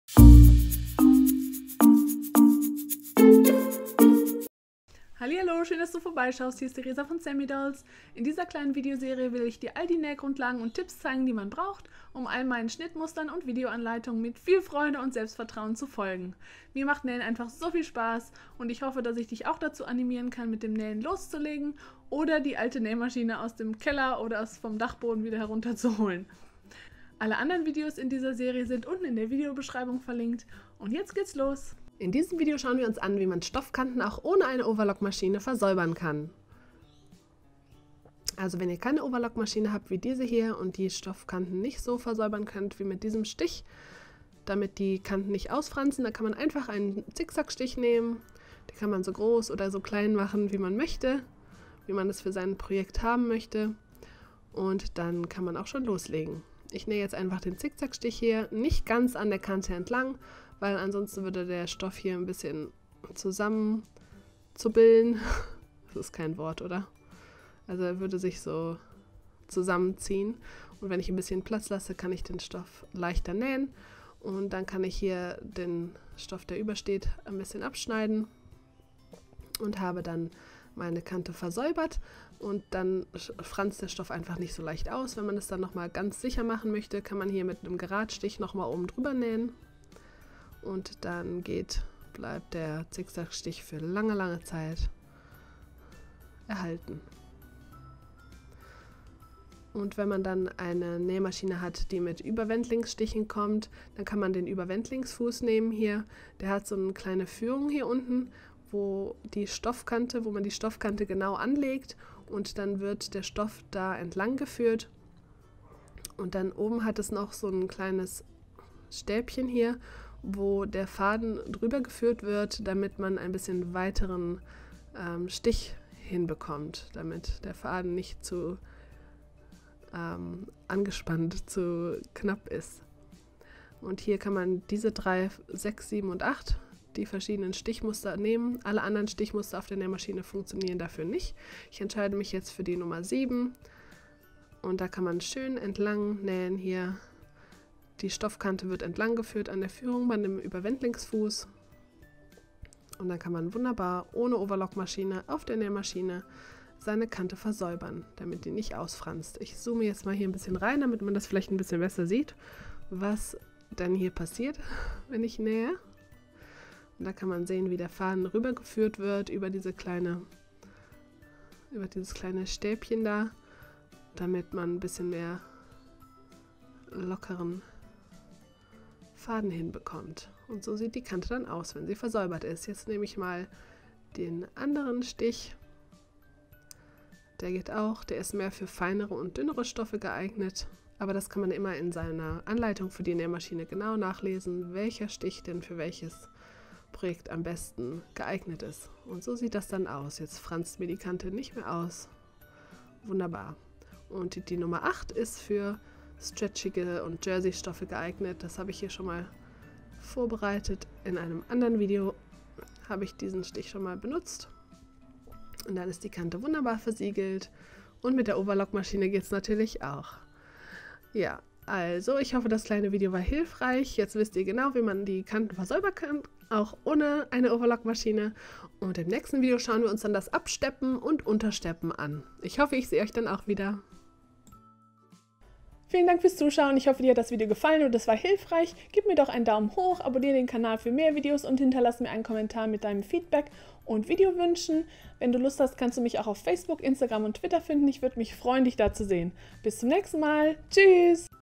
Stoffkanten versäubern ohne Overlock Maschine • Nähen lernen für Anfänger Serie
In diesem Video schauen wir uns an wie man die Stoffkanten versäubern kann, damit der Stoff nicht ausfranst - ganz ohne Overlock Maschine.

💗 Hier ist der Link zum Shop (eBooks, handgemachte Unikate) oder